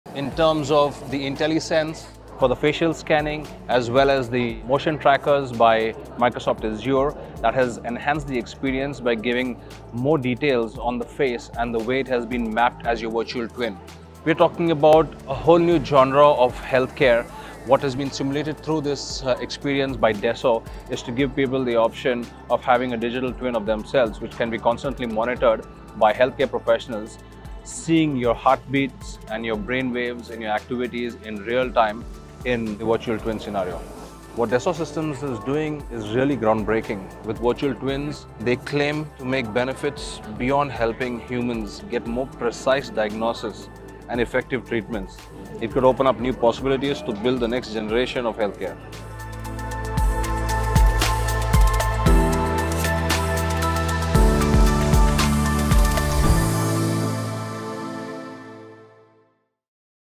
The Next Generation 3D Volumetric LED Virtual Twin - Dragon O
We've seen the immersive experience of The Virtual Twin at Dassault Systemes CES delivered by LedPulse on the Volumetric LED display, now let's check the advanced features that have opened new possibilities for healthcare industries in terms of simulating in realtime accurate diagnosis of brain waves, heartbeats, weight, etc. through the Volumetric Virtual Twin at CES 2023 hosted by Sujoy Cherian, creative entrepreneur & The Technologist.